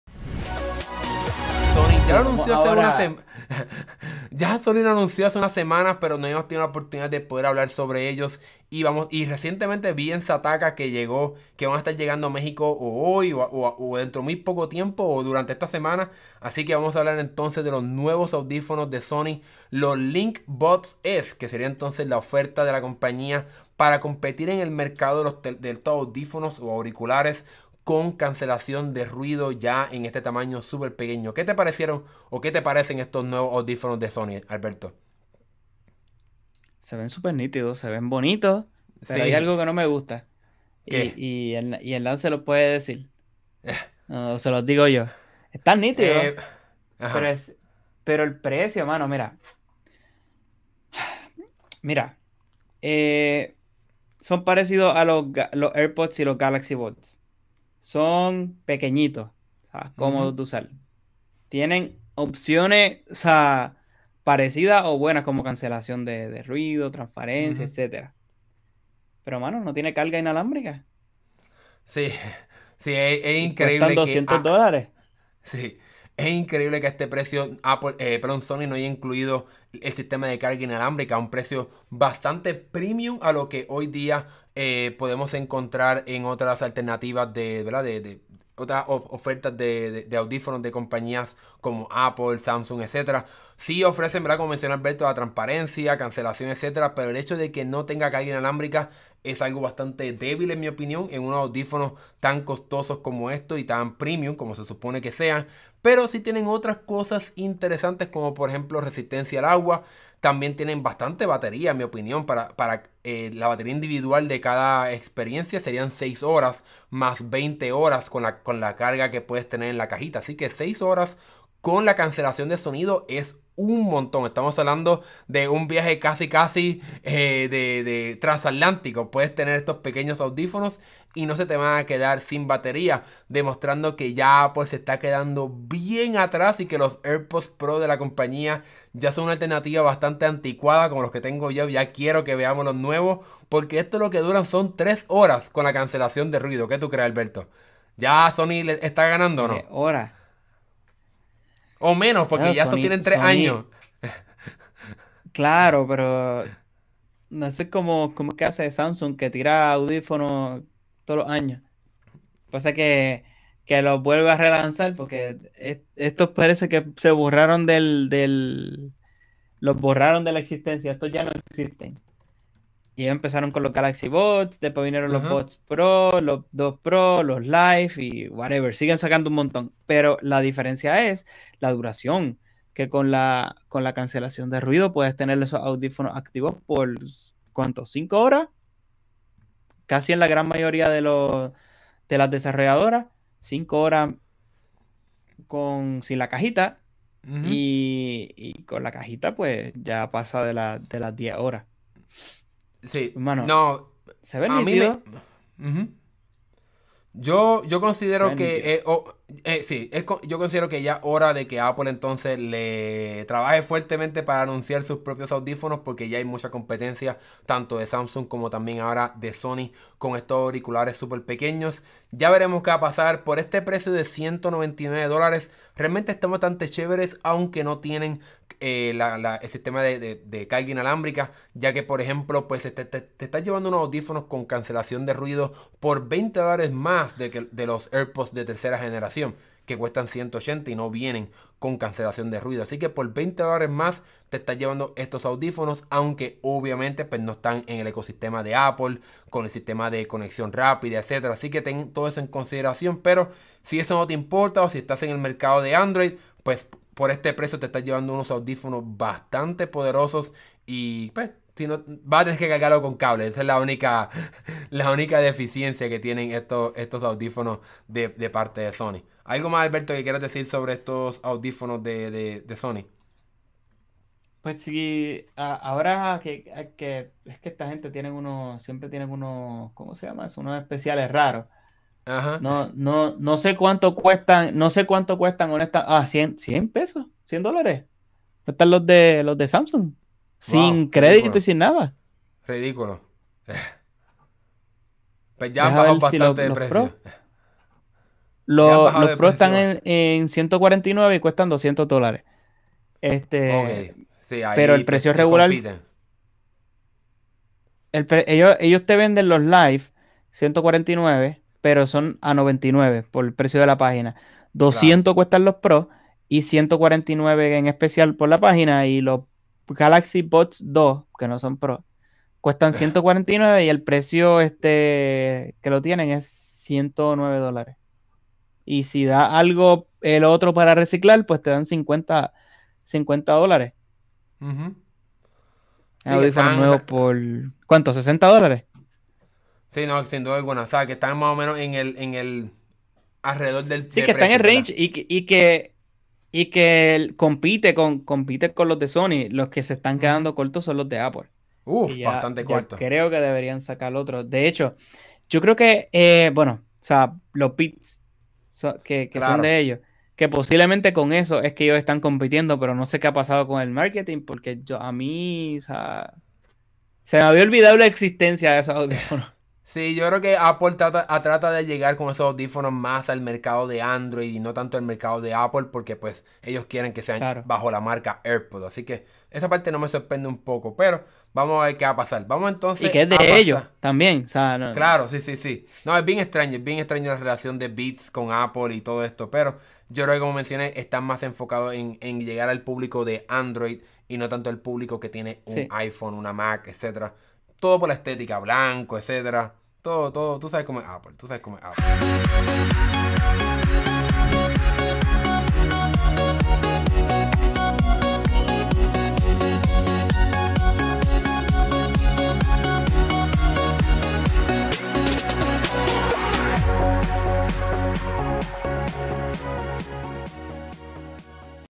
Sony Link Buds S - PUEDEN competir con Apple y Samsung?
Te contamos todo sobre los nuevos audífonos de Sony, los Link Buds S y cómo se comparan con las ofertas de Apple y Samsung con sus respectivos auriculares inalámbricos.